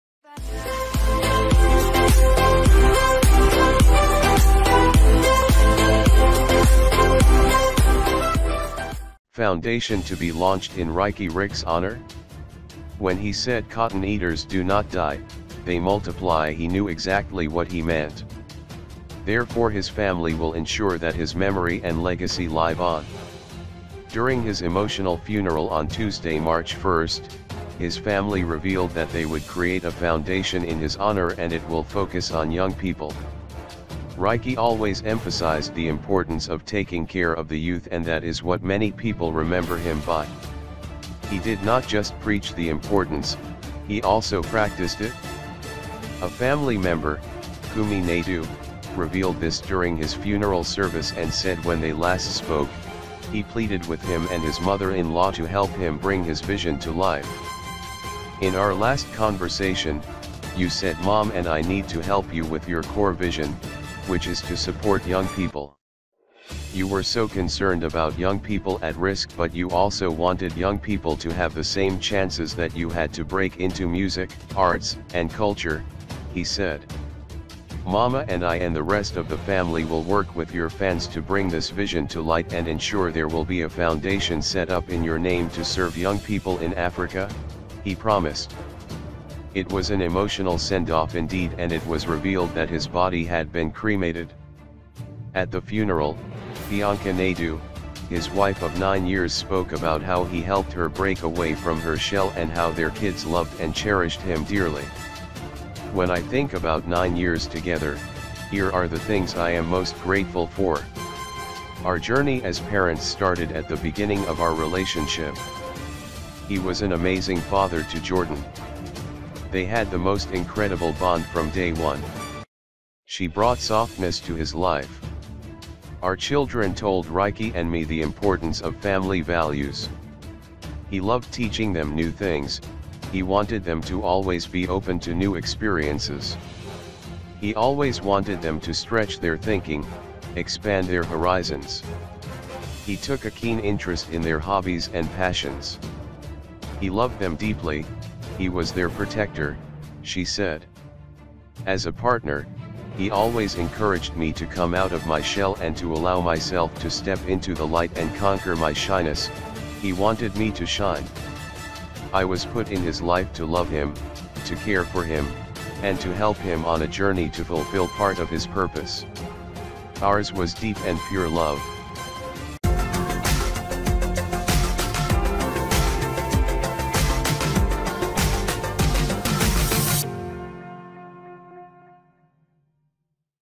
Foundation to be Launched in Riky Rick’s Honour
When he said ‘Cotton Eaters do not die, they multiply’ he knew exactly what he meant. Therefore his family will ensure that his memory and legacy live on. During his emotional funeral on Tuesday, March 1, his family revealed that they would create a foundation in his honor and it will focus on young people.